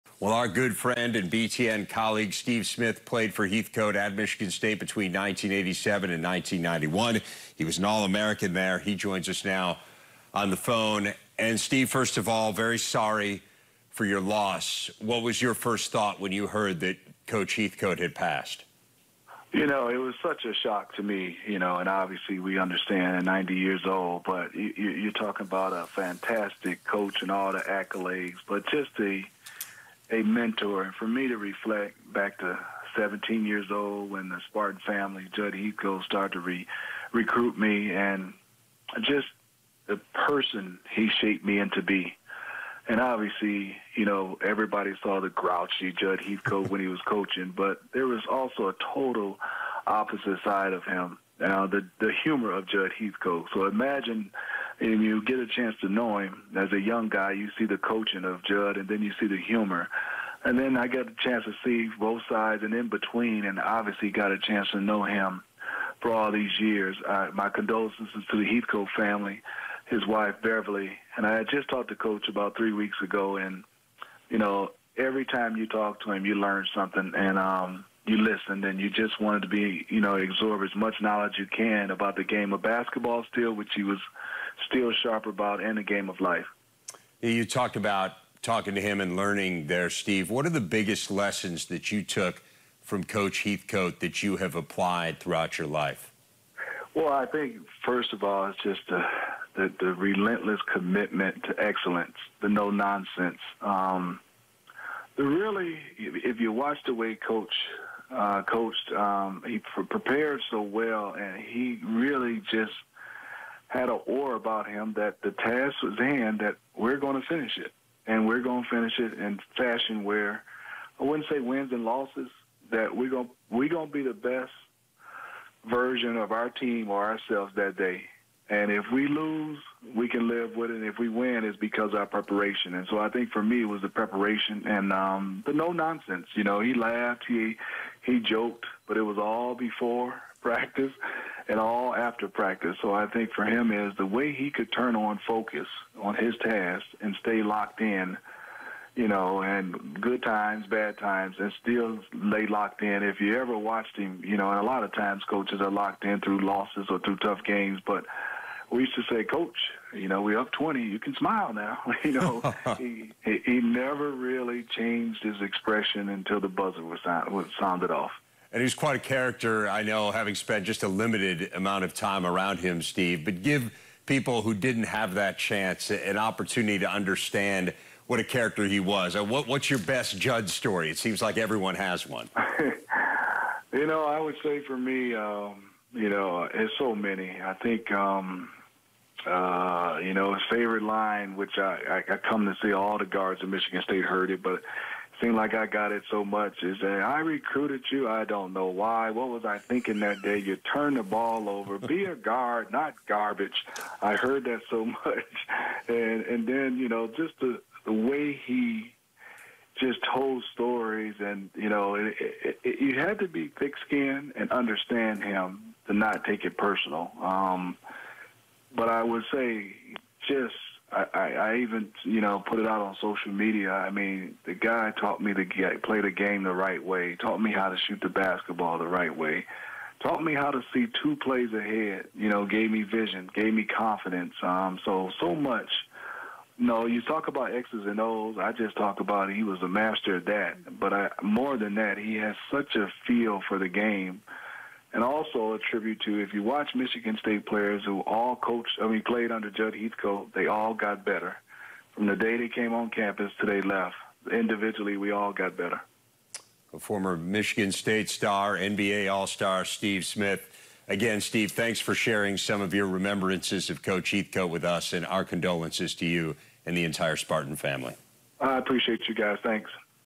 Steve Smith Remembers Jud Heathcote
Steve Smith, former Michigan State player under Jud Heathcote, talks with Dave Revsine to reflect on his former coach who recently passed away.